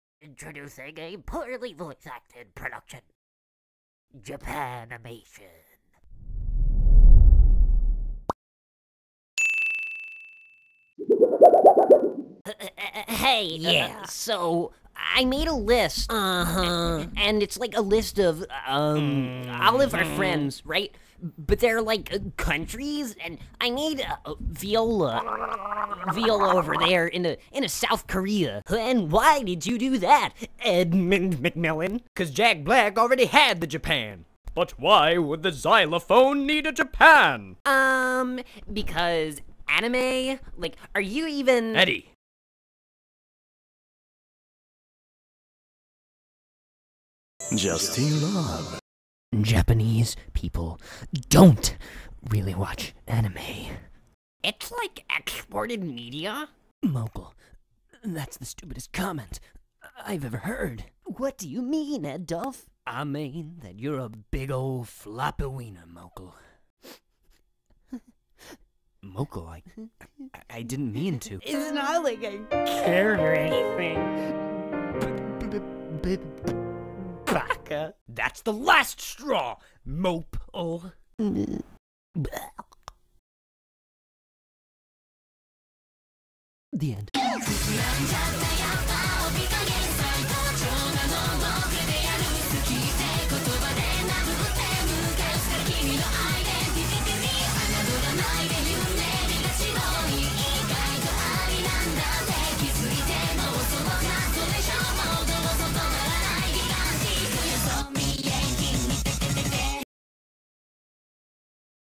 Japanese People Don't Watch Anime
"Japanese people don't really watch anime... it's like exported media"
     ~Mokel Boble